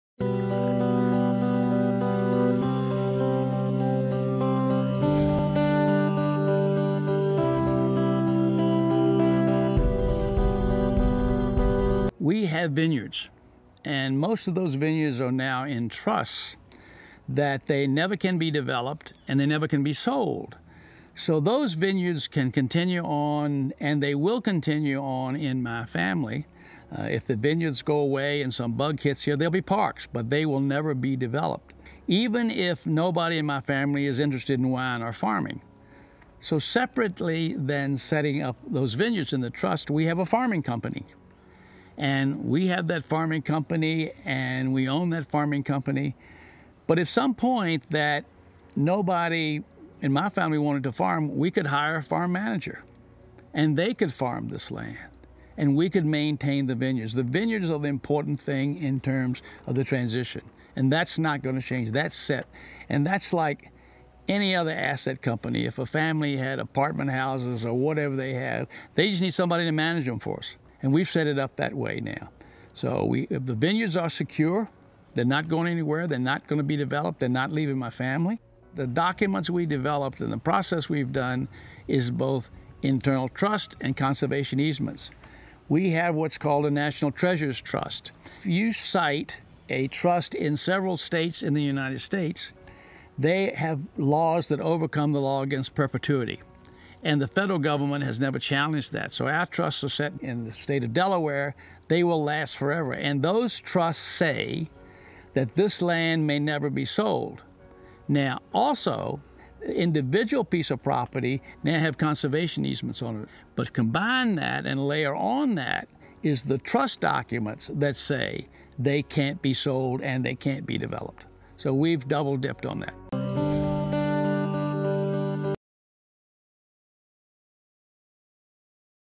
Wine Business Asset Strategy #2: Land Trust & Conservation Easements | Business of Wine Video Series
Andy Beckstoffer, founder of Beckstoffer Vineyards, on preserving wine business assets.

This video is an extract from the 13-part The Business of Wine Video Series provided free for educational purposes by the Wine Business Institute in the School of Business and Economics at Sonoma State University.